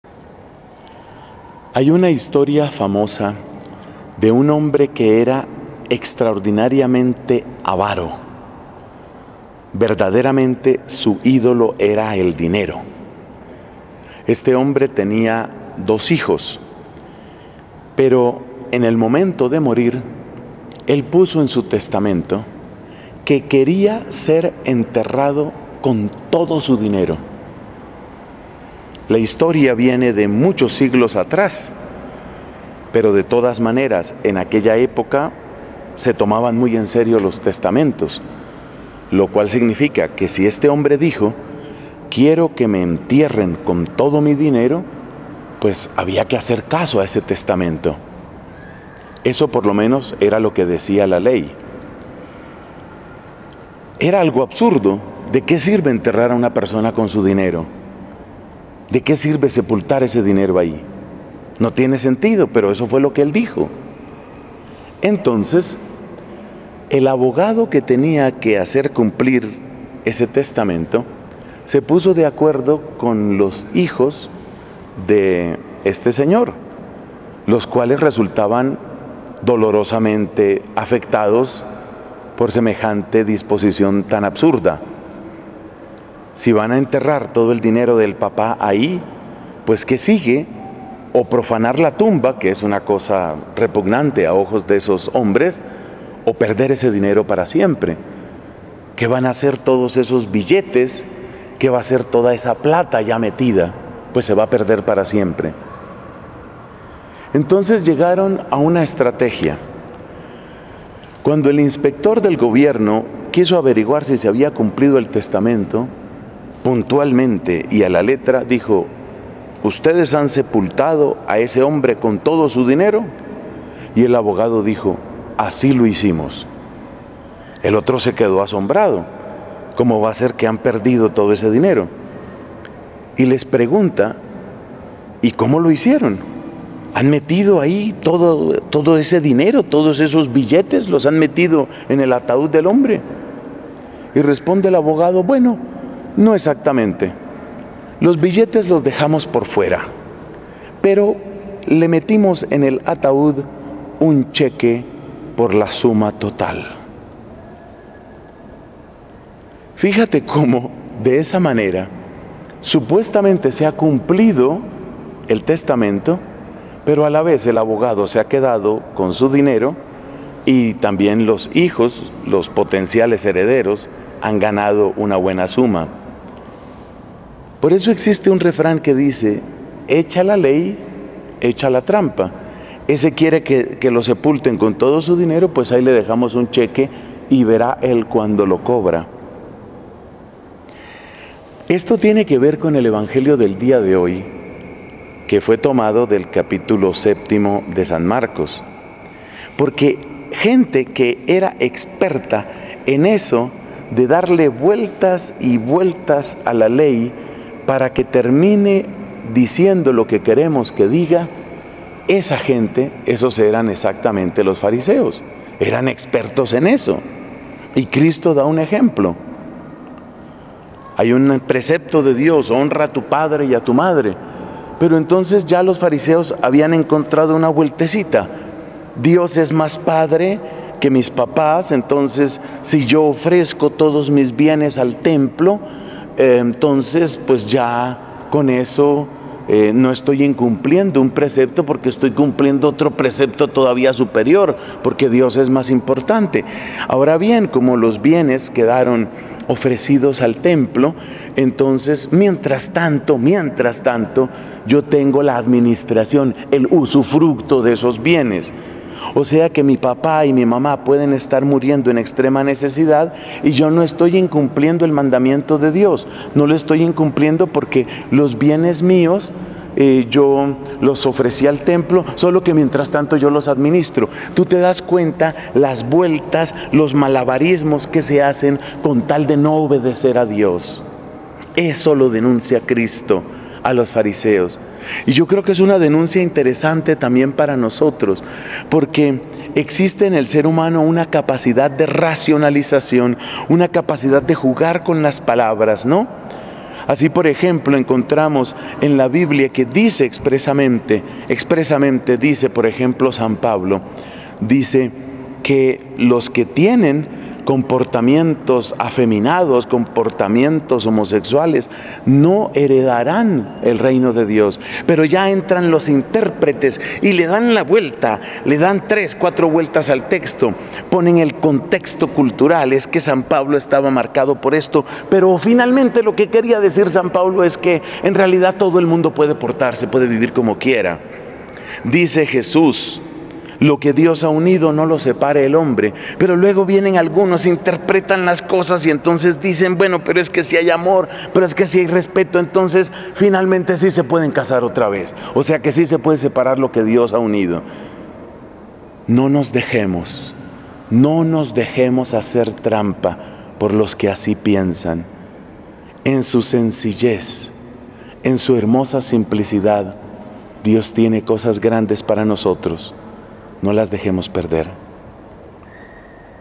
LA GRACIA del Martes 9 de Febrero de 2016
Dios tiene grandes planes para nosotros, no los dejemos perder por seguir personas que mal interpretan y acomodan la Palabra de Dios para su propio beneficio.